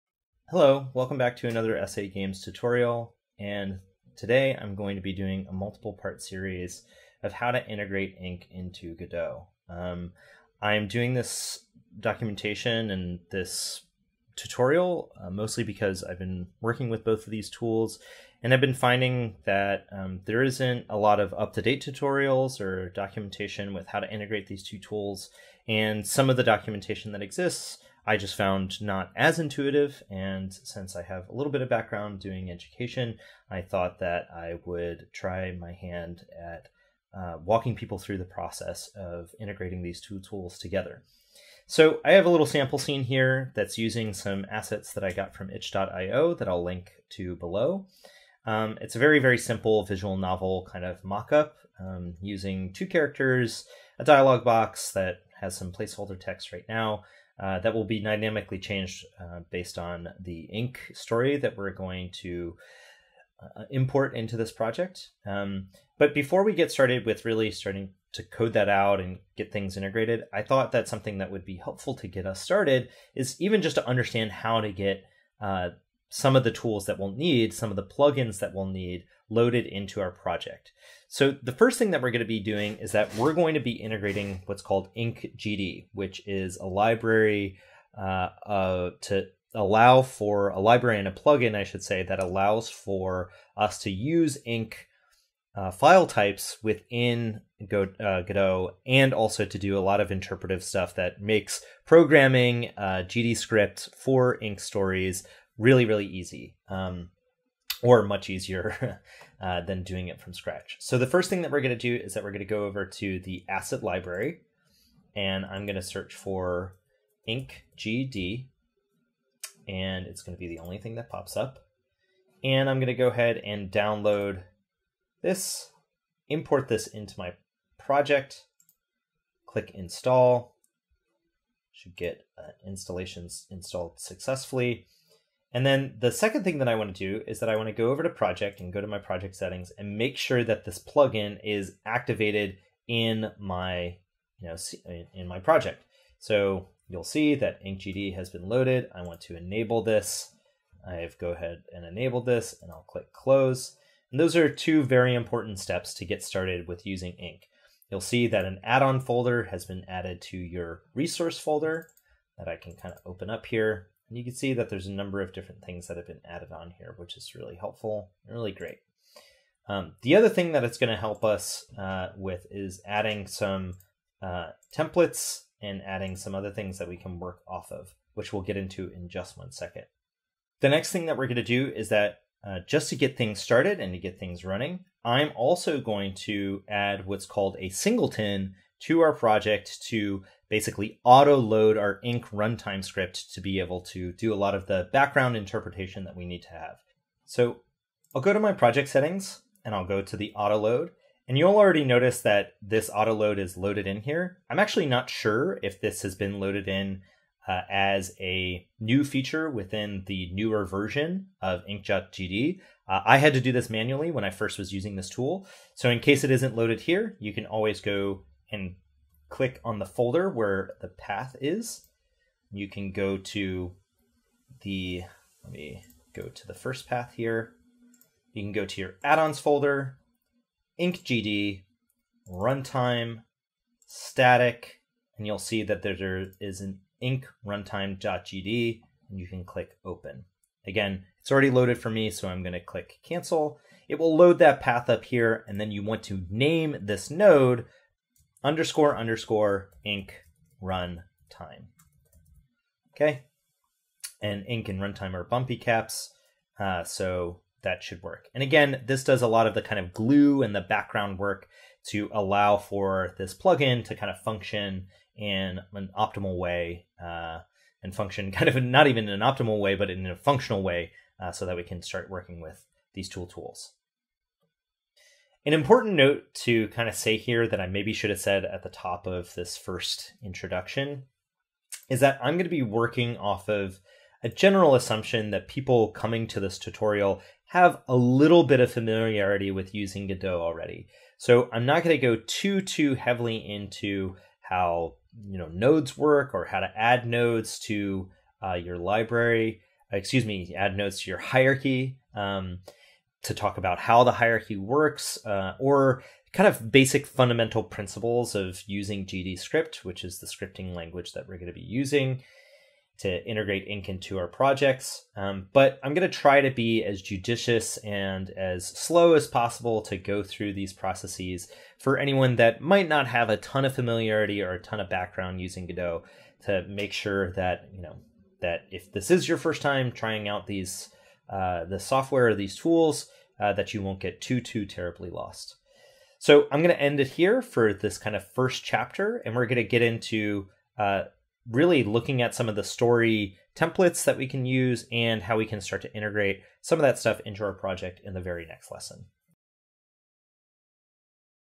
Godot & Ink: Chapter 0 — Getting Started
This tutorial is a hands-on practical demonstration of using Ink with the Godot game engine.

This project uses the inkgd plug-in developed by
Frédéric Maquin: